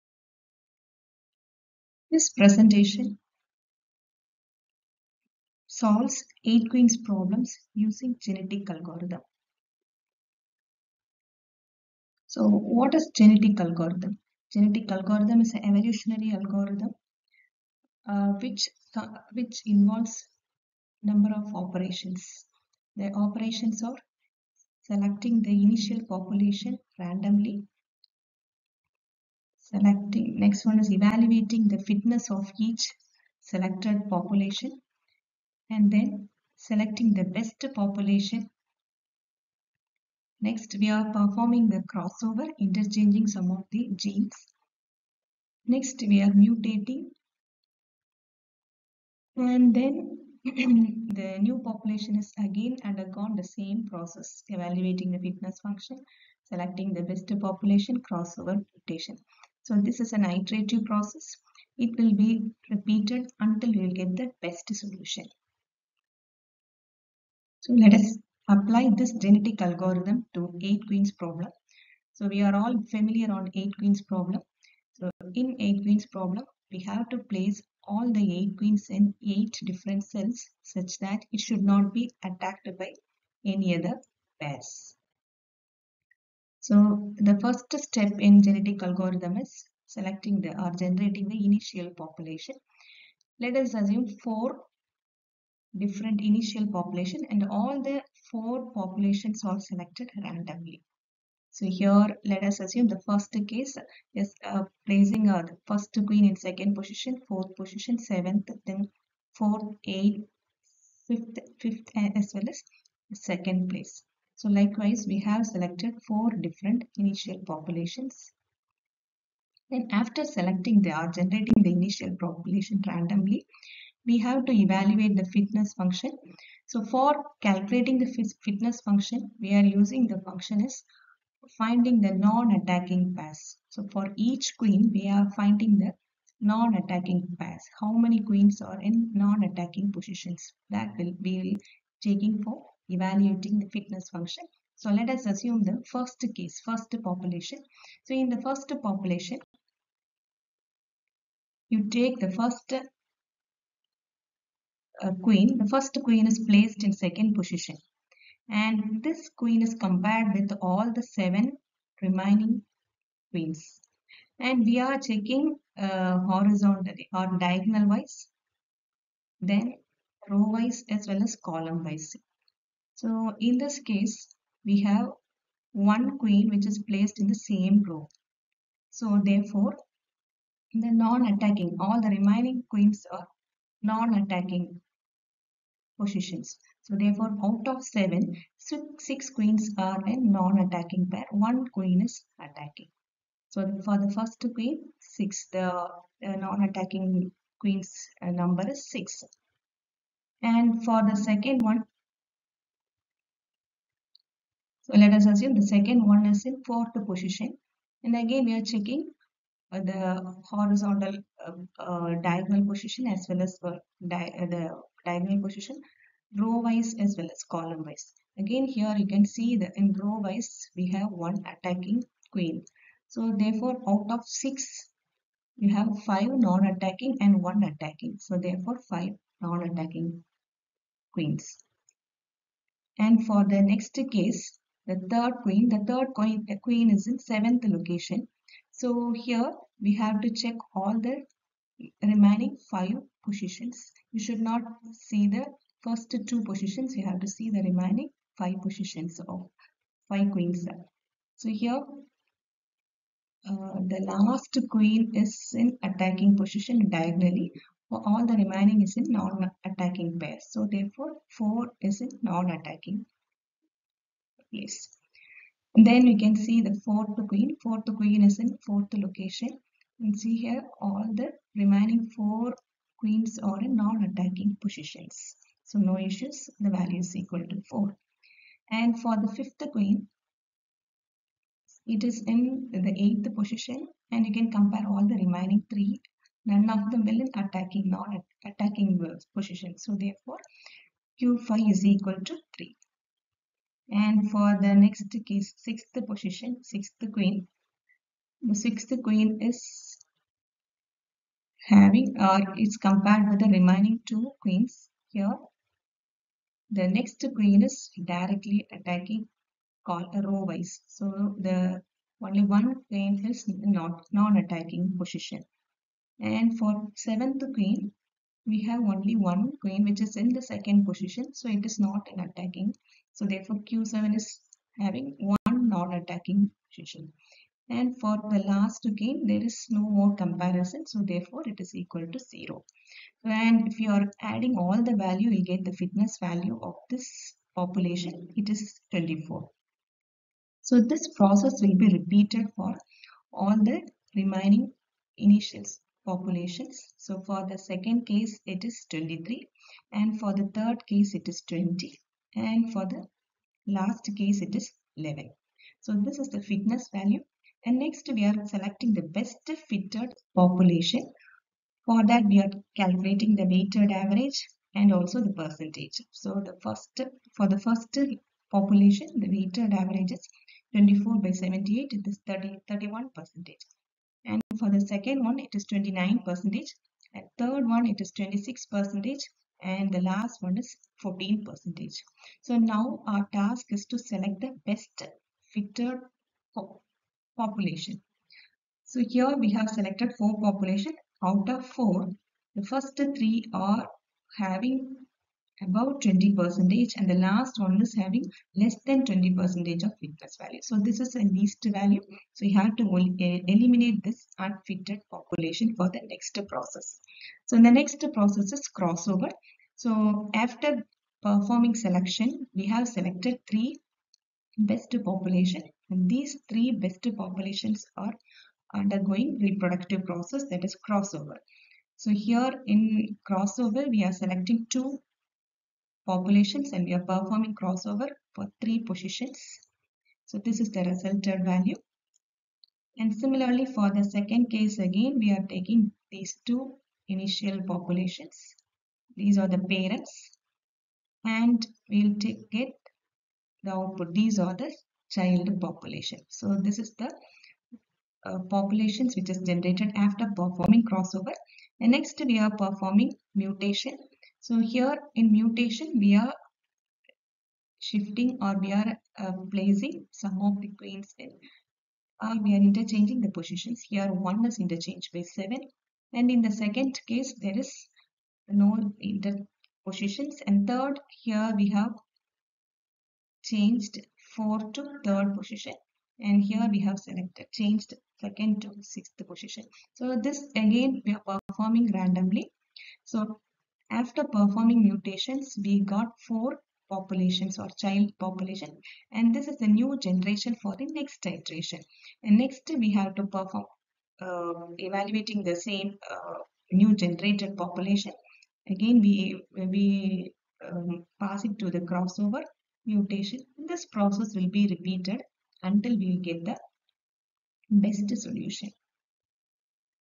8 Queen's problem using genetic algorithm
Solving 8 Queen's problem using genetic algorithm